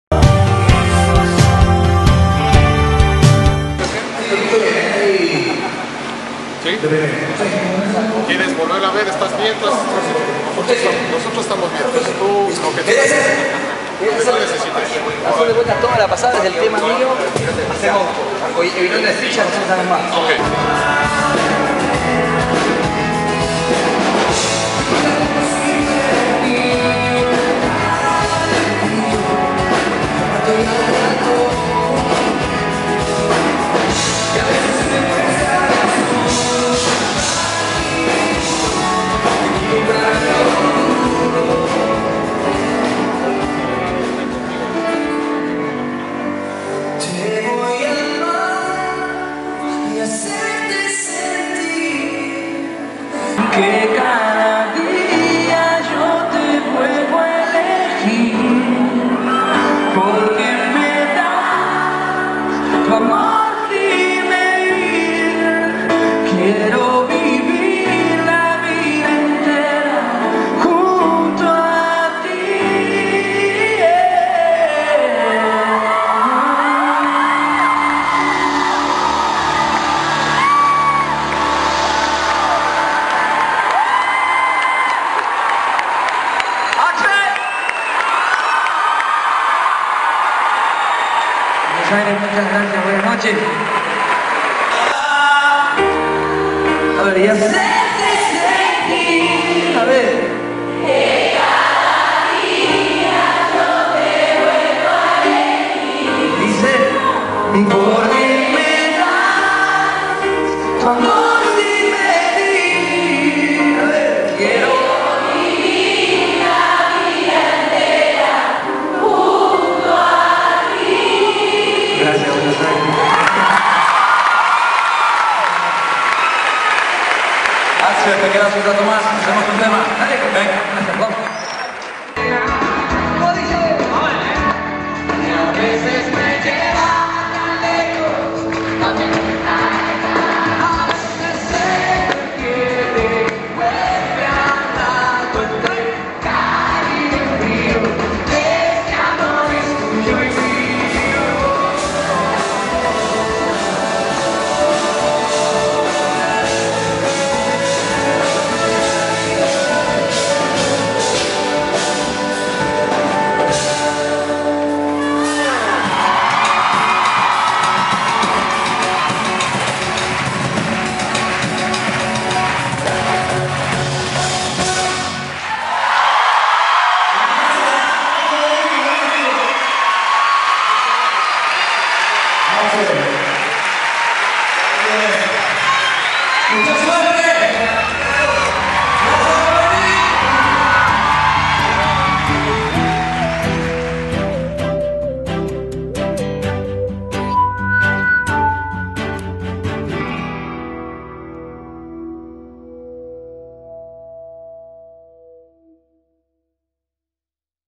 AXEL CANTÓ CON FRANCO DE VITA EN EL GRAN REX
Axel fue invitado especialmente por el cantante venezolano Franco De Vita para participar del show que brindó el pasado 1º de Mayo en el Teatro Gran Rex.